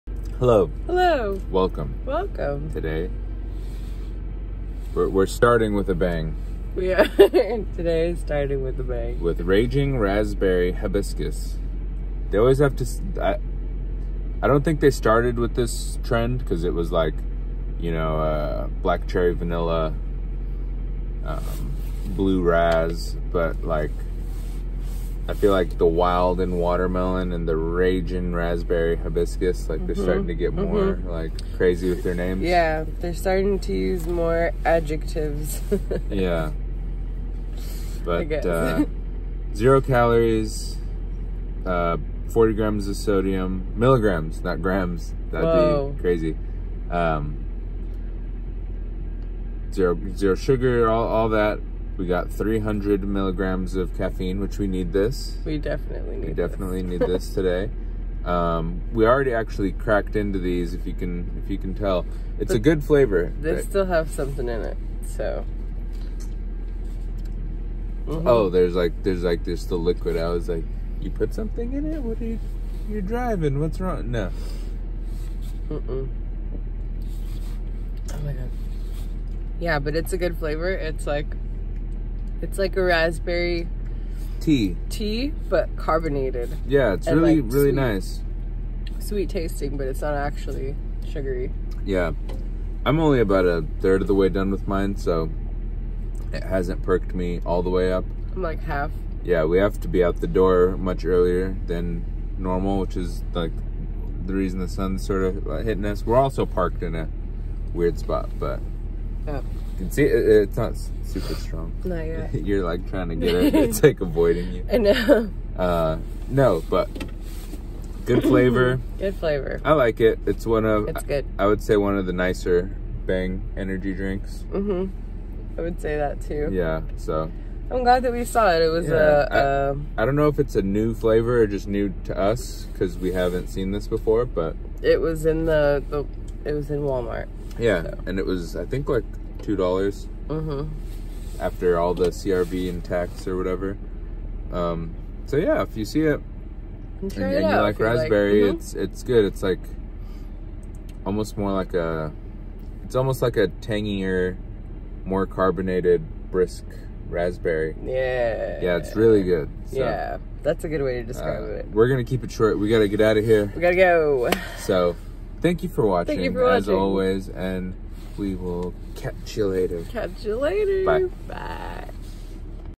Bang Raging Raspberry Hibiscus 🌺
Our morning fuel of choice today is Raging Raspberry Hibiscus from Bang!